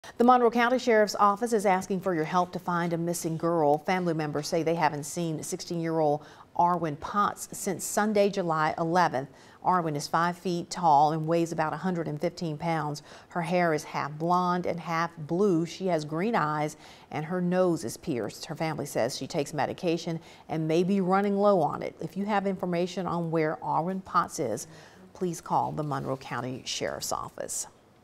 Missing Monroe County Teen
Monroe County Sheriff's Office has released the name of a missing teen girl.  Sixteen year old Arwen Potts has not been seen since July 11th.